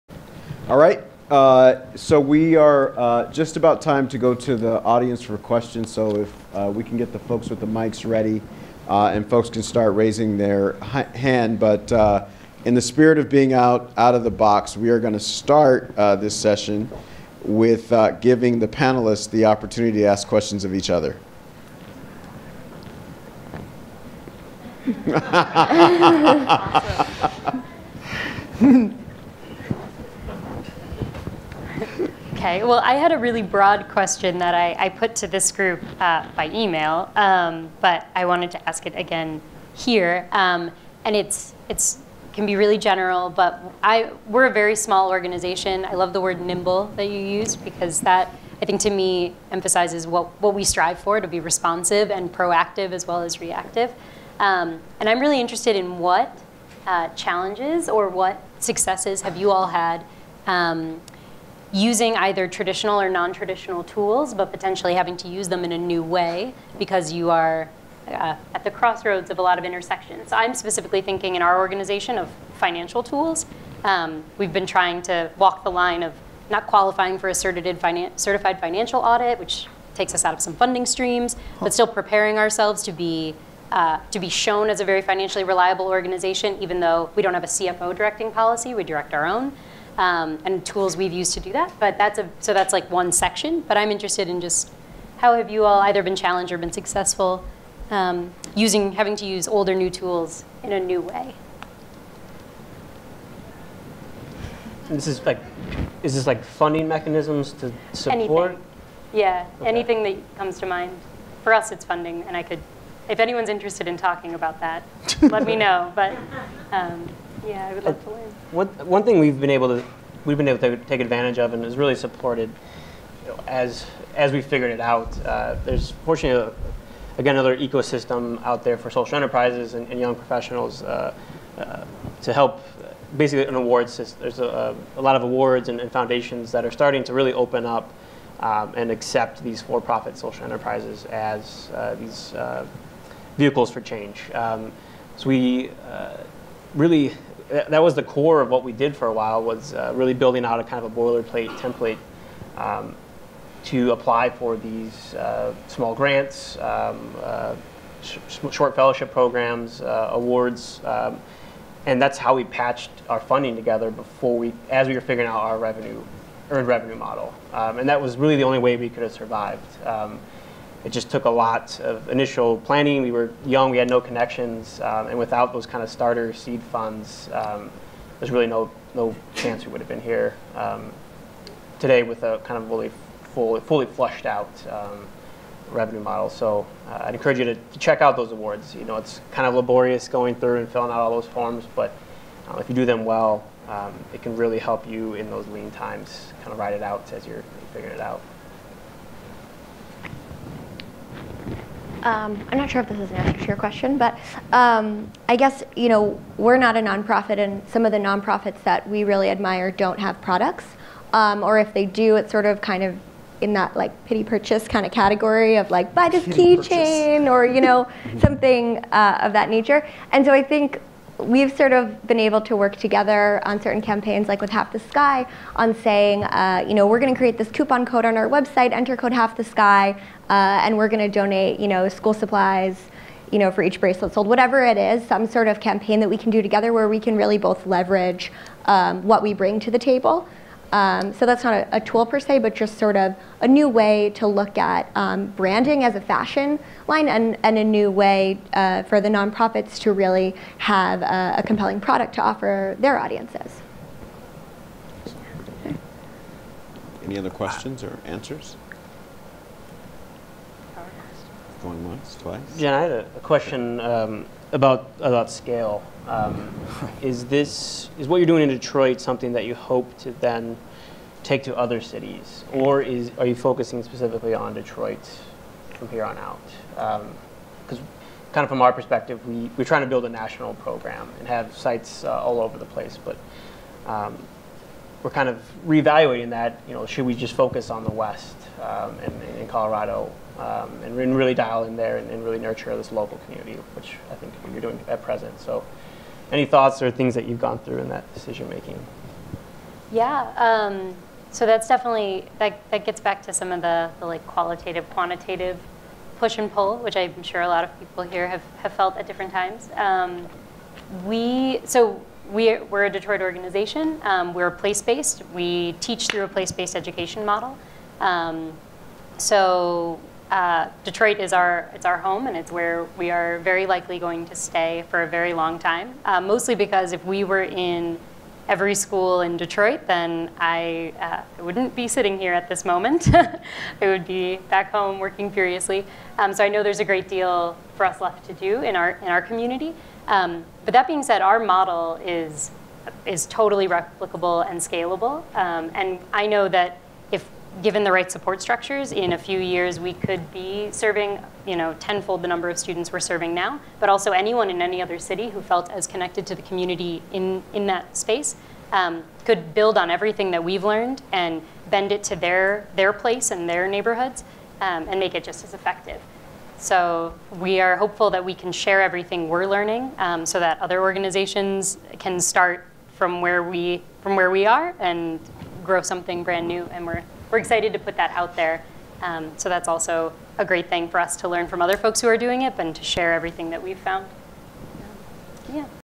A New Generation Envisions the Future: Discussion between Panelists
On October 30, 2013, Public Interest Projects convened "Breaking Out: Reinventing Philanthropy in the 21st Century." The second panel explored how engaging Millennials is key to creating an more just and equitable world. Tapping into a different skill set that includes digital fluency, creativity and passion, this panel of problem-solvers will explore their ideas and the model they have created to tackle the world's problems and create change.